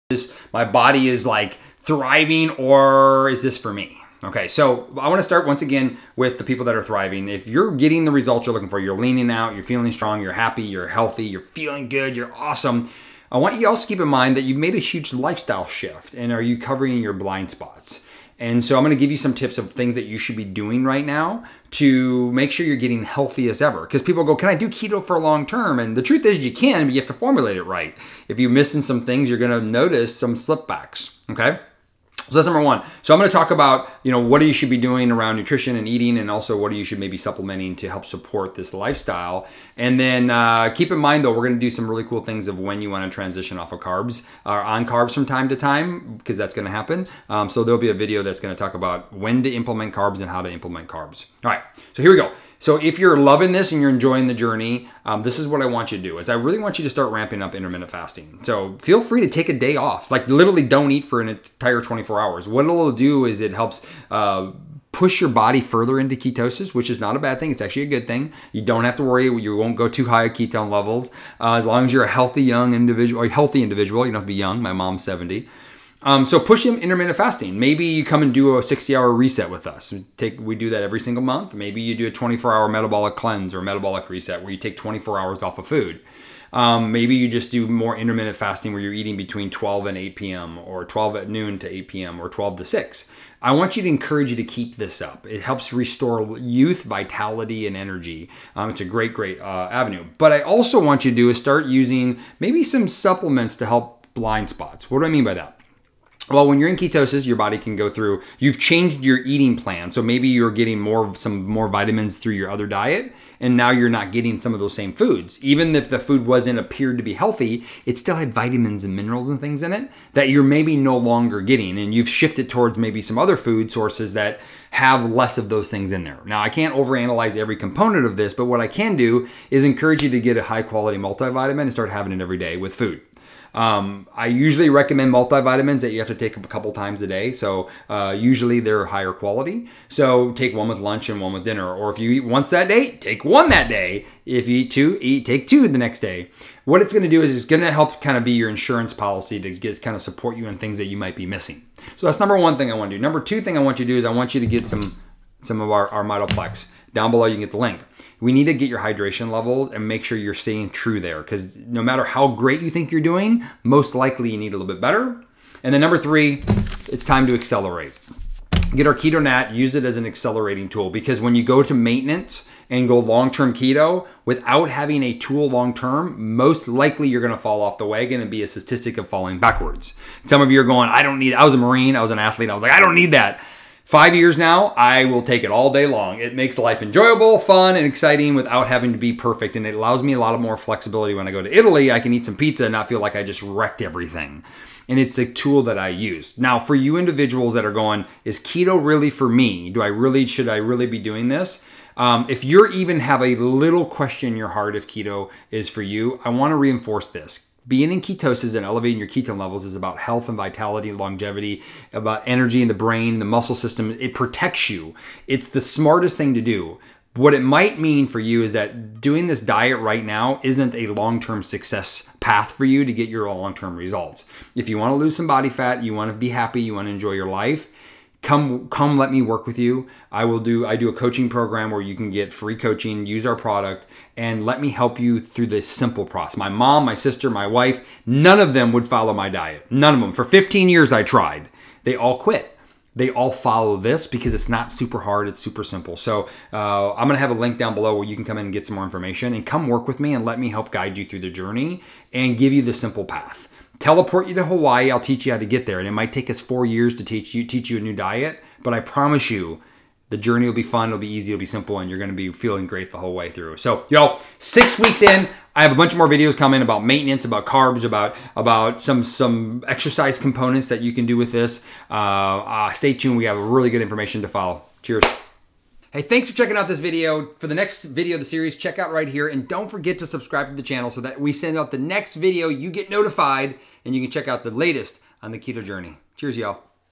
WEEK 6- Keto Transformation Guide | MAXIMIZE YOUR KETO JOURNEY | Dustin Schaffer The Keto Pro
If you’ve been considering options to improve your health, you may have heard about the popular ketogenic diet — also known as the keto diet. But did you know that today’s latest diet trend isn’t all that new?

Unlike its predecessor, the Atkins diet, the Ketogenic (or Keto) diet not only restricts carbohydrates, but also protein. This aims to induce a state of fat breakdown into fatty acids, which become the fuel that cells use for energy instead of glucose.
But after a few weeks of following this diet, are you thriving or not?